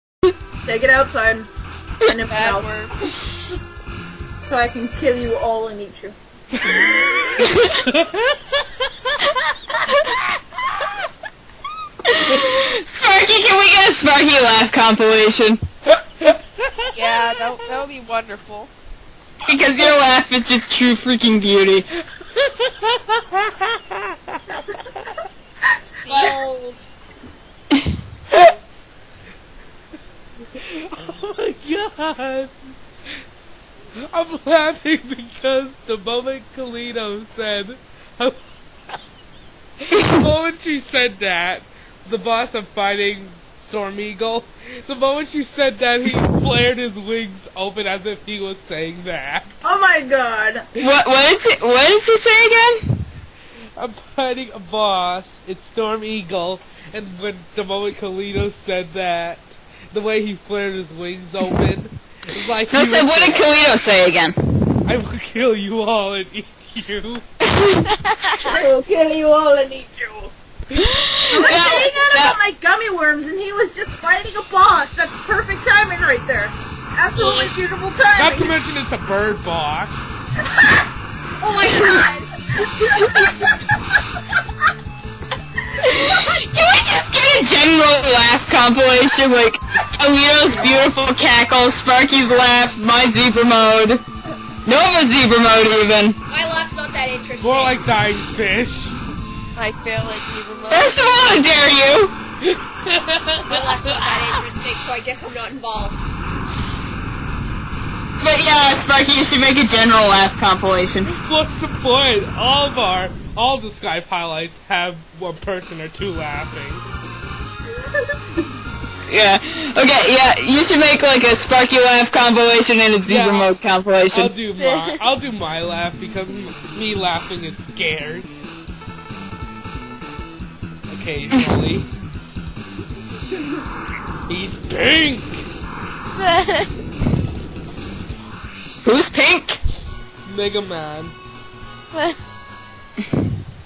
Skype Highlights 9 | Gummy Worms
Perfect timing as Kaliedo talks about gummy worms. No laugh compilation yet. We'll get to that.
STARRING: Syo, Sparky and Guest Stars Kaliedo, Cereal, and Nova (i think Barbs might be there too, but I'm not entirely sure)